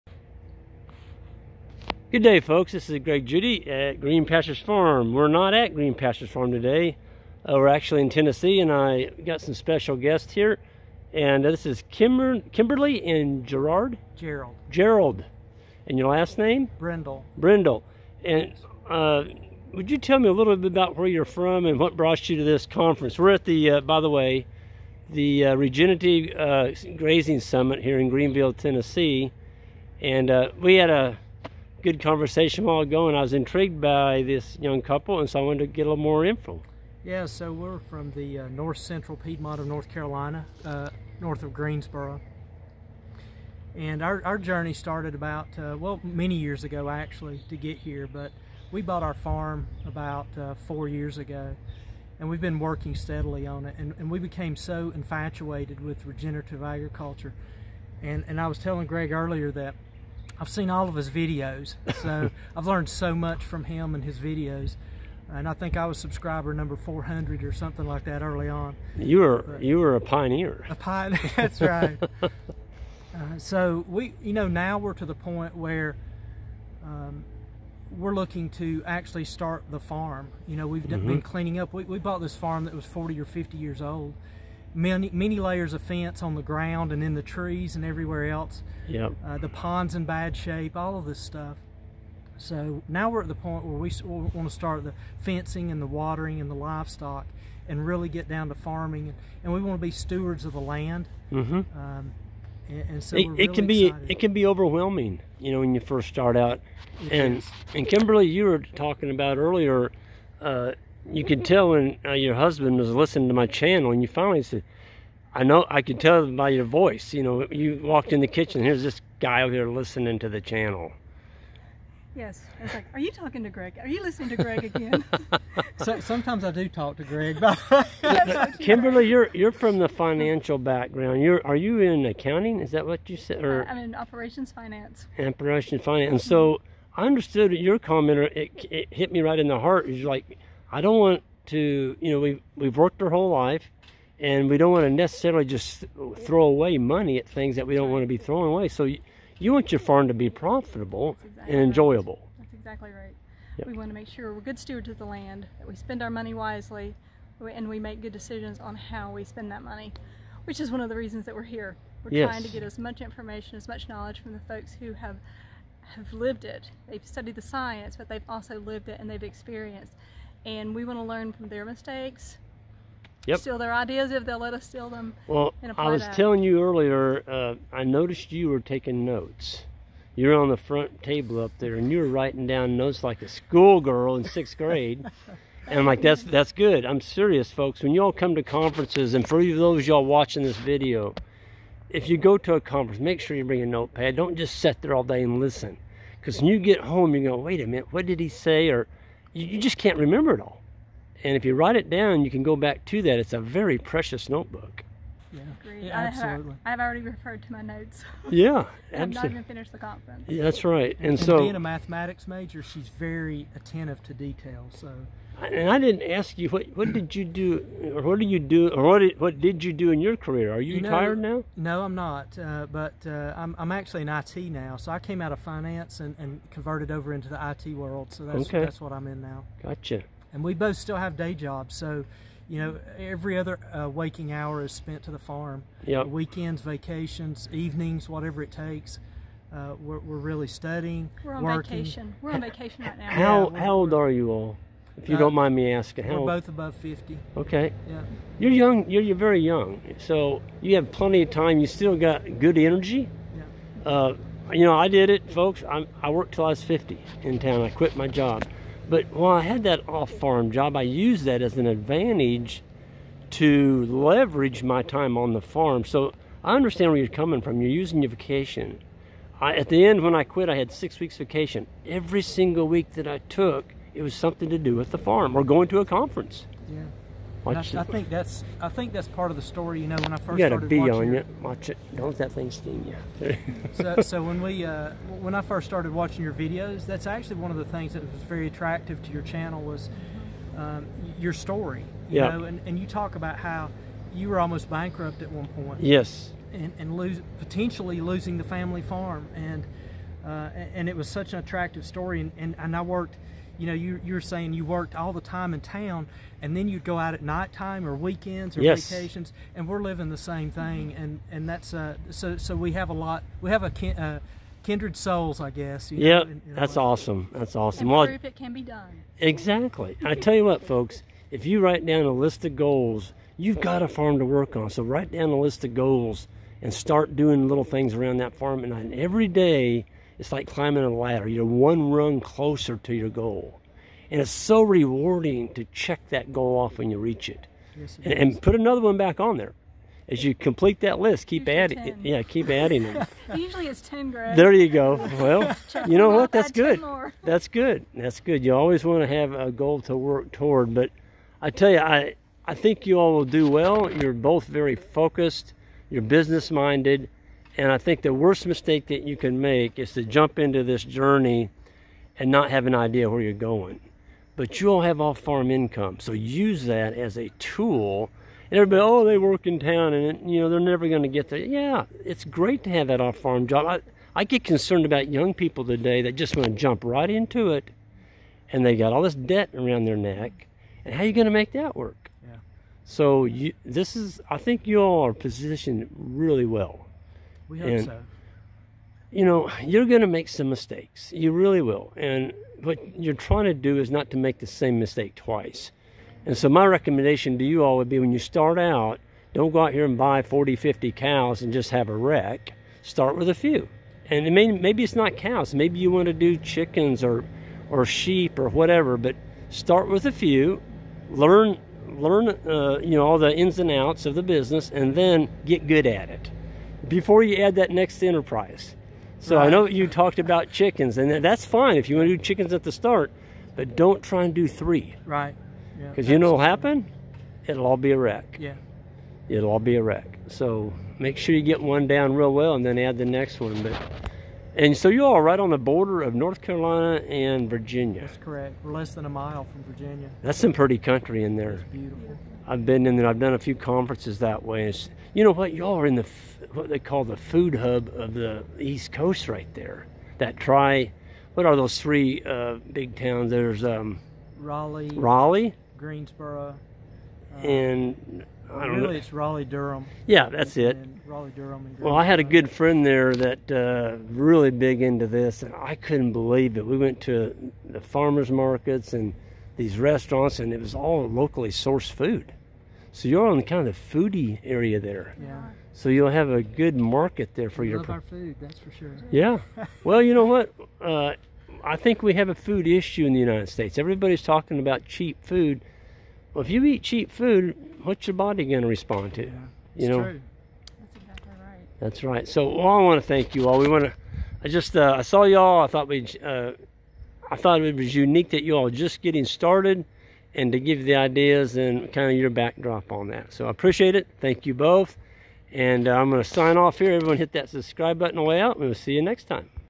New farming couple getting started on their own farm.
New farming couple getting started on their own farm. It is refreshing to see new farmers being drawn back to the land to begin their regenerative grazing journey. Passionate folks wanting to control their food and live a healthy life.